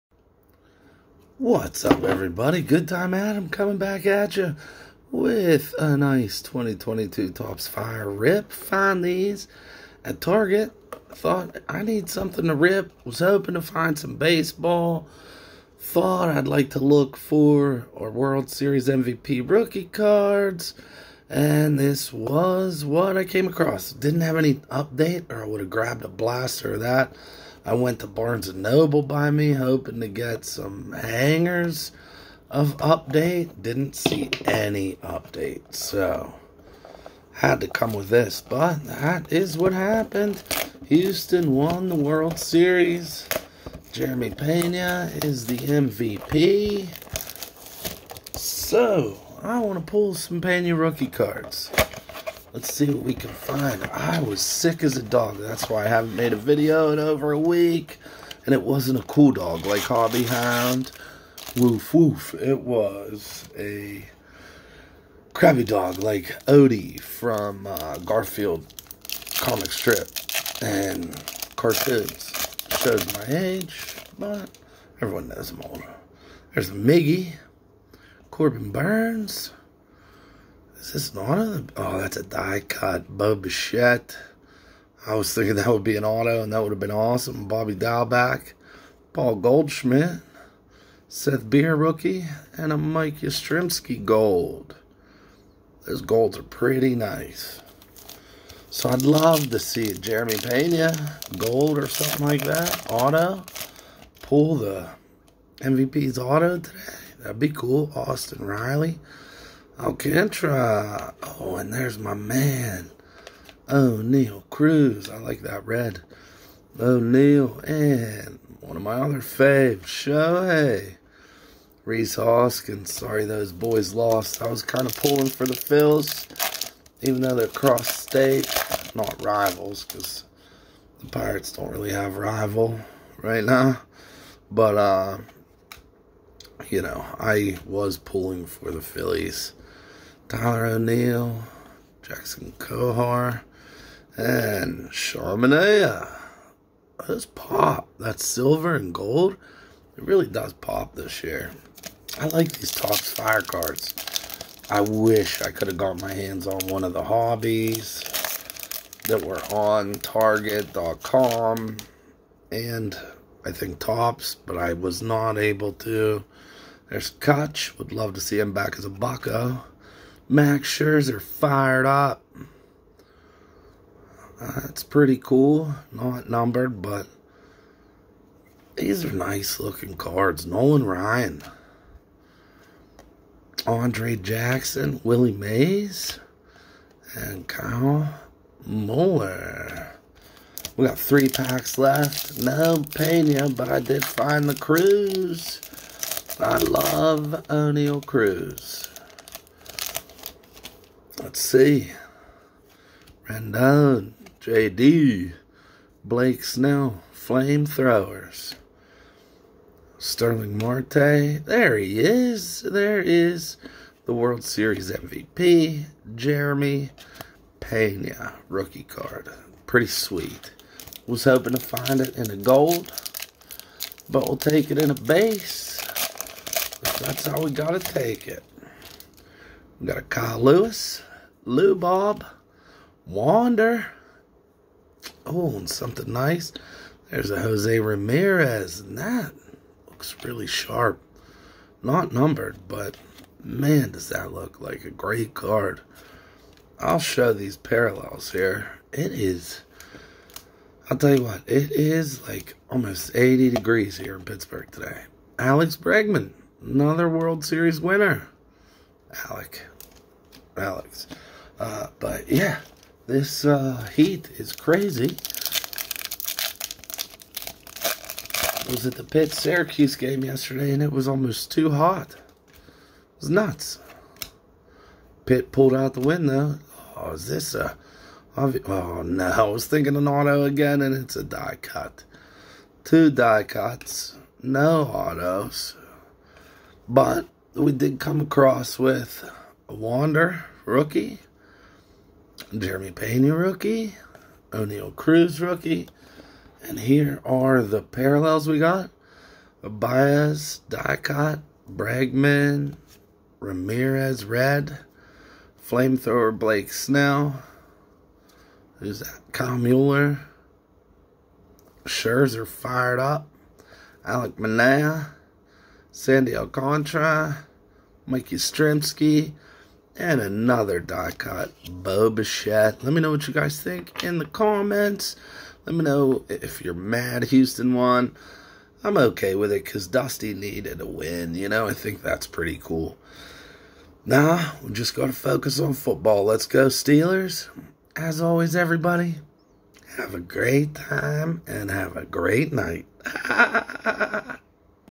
Topps 🔥 2022 Blaster Box
Looking for some rookies, especially the World Series MVP Rookie Cards.